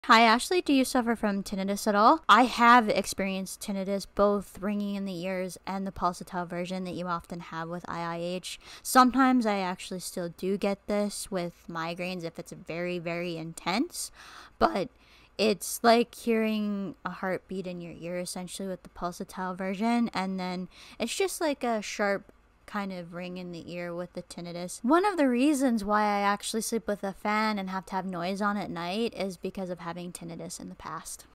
Have I Experienced Tinnitus | Responding To Your Comments | Part 4
I was asked if I've ever had tinnitus in one of my comments.

Want me to respond to your comment? Leave them in the comments of this video!